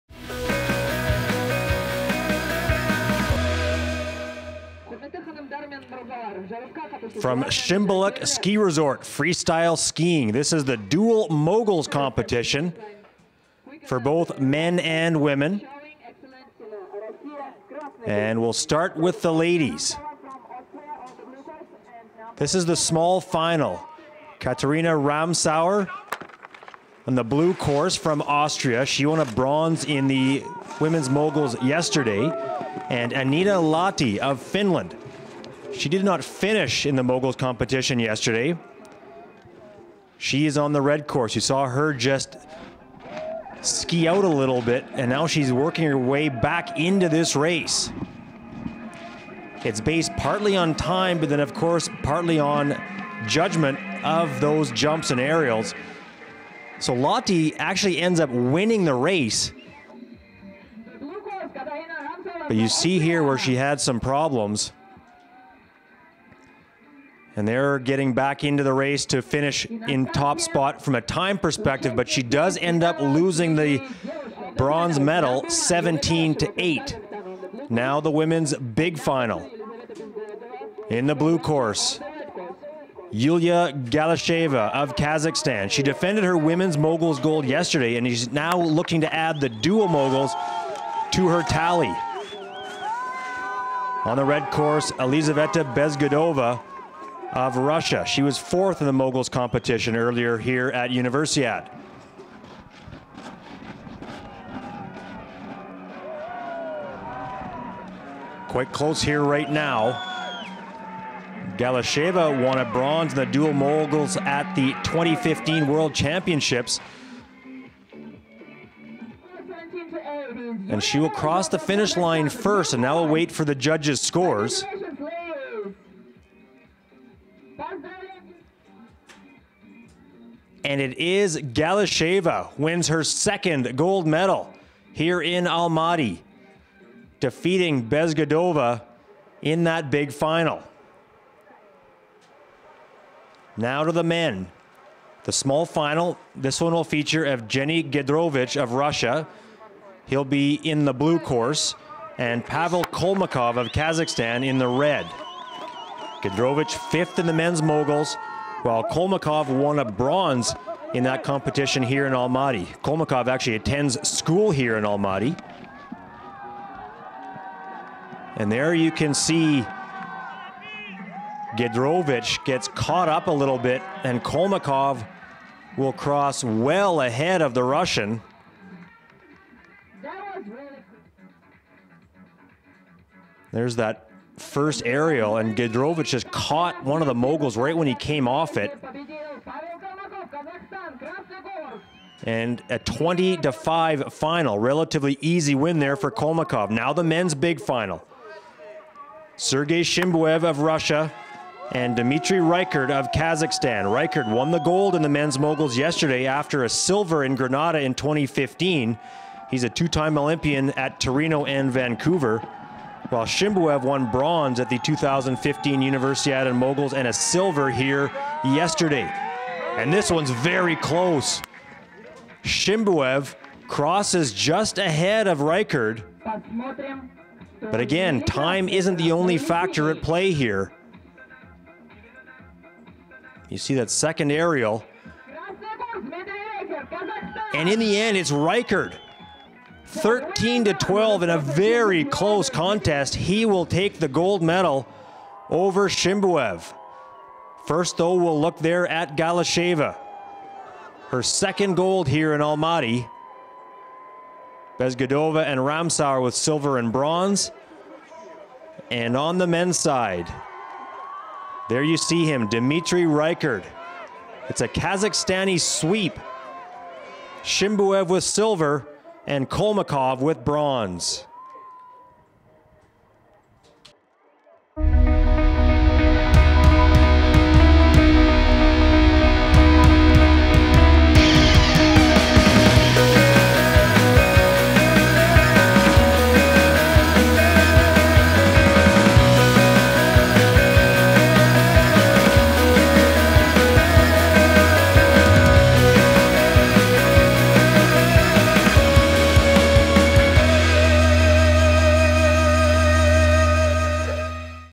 Freestyle Skiing Ladie's and Men's Dual Moguls - 28th Winter Universiade 2017, Almaty, Kazakhstan
ALMATY – 3 February, the Dual Moguls competition took place in Shymbulak Ski Resort, outside Almaty. It was a double gold for the host nation as both in the women’s and men’s competition Kazakh athletes excelled.

In the women’s race 13 athletes from 7 countries competed. Yuliya Galysheva (KAZ) beat Elizaveta Bezgodova (RUS) in the final race while Austrian Kataharina Ramsauer beat Anniina Lahti from Finland in the small final, pocketing the bronze.

In the men’s race 15 athletes from 6 countries competed for the gold. It was Dmitriy Reikherd, also from Kazakhstan, who finally defeated Sergei Shimbuev from Russia in the gold quest. The bronze medal went to another Kazakh athlete, Pavel Kolmakov, who beat Russian Evgenii Gedrovich.